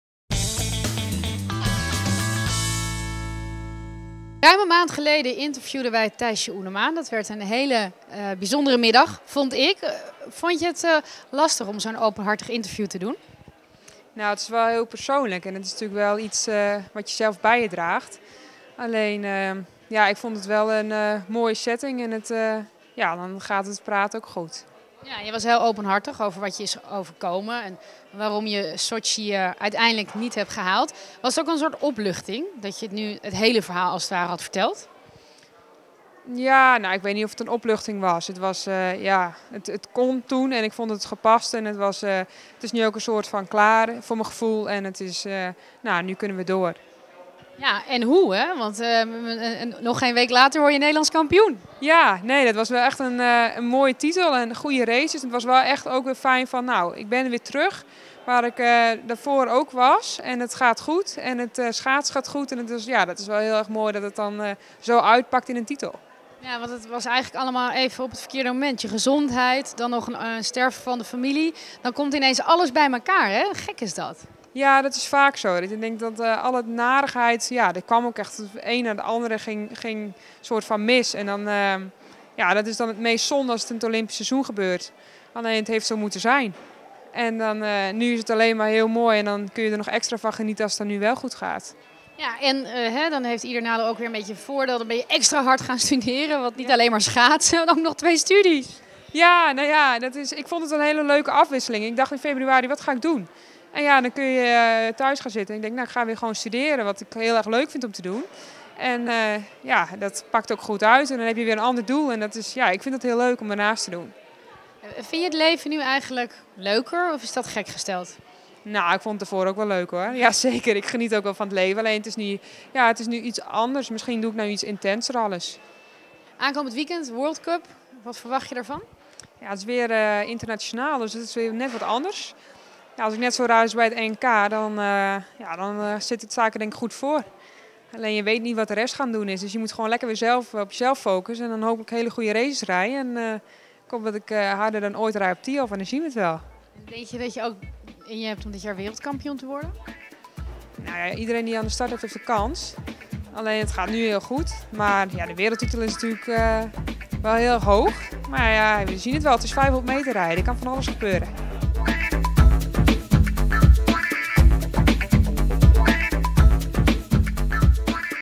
Lancering Helden 25: Thijsje Oenema
Barbara Barend in gesprek met schaatsster Thijsje Oenema tijdens de lancering van de nieuwe Helden, editie 25.